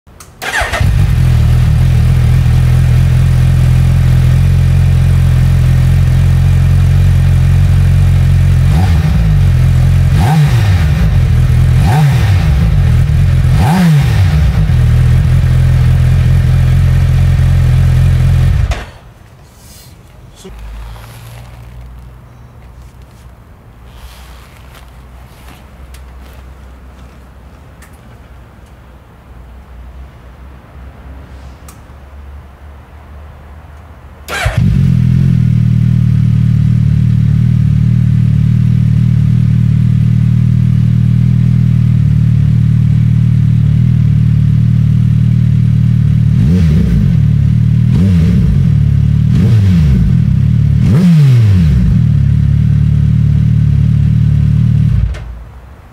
Akrapovic Slipon - with baffle vs without baffle || 2021 HAYABUSA
A speakers with good bass required !

00:00 Baffle O
00:21 Baffle X